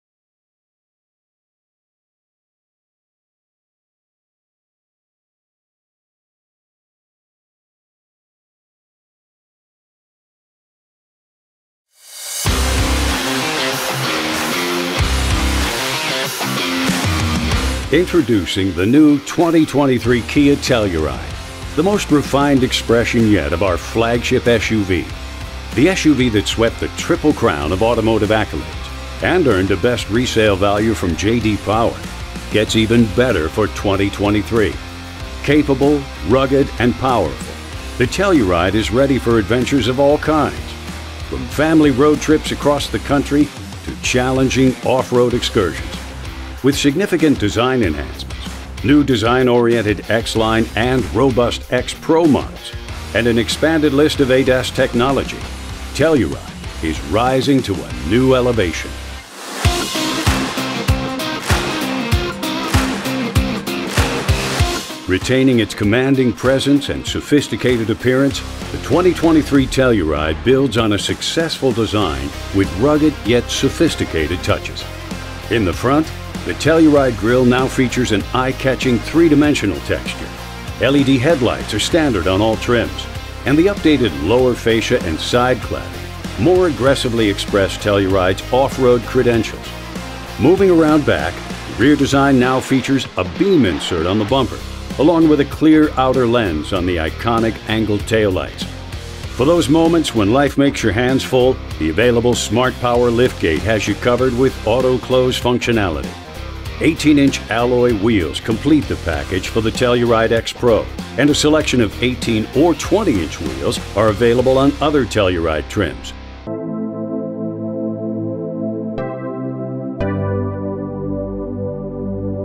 2024 Kia Telluride: Enhanced Style and Ruggedness for Texas Trails | SUV Review.
Discover the impressive exterior design and enhanced ruggedness of the 2023 Kia Telluride in this exciting SUV review. As one of the most sought-after and award-winning SUVs in the US, the Telluride returns to Texas with notable upgrades and two new adventurous trim levels. Get a closer look at its restyled features, including revised headlamps, a refreshed grille, and new wheel designs. Step inside the spacious and upscale interior with dual 12.3-inch display screens and updated color packages. Experience the power, capabilities, and advanced safety features of the 2023 Telluride, coming to showrooms this fall. Don't miss out on this trailblazing SUV!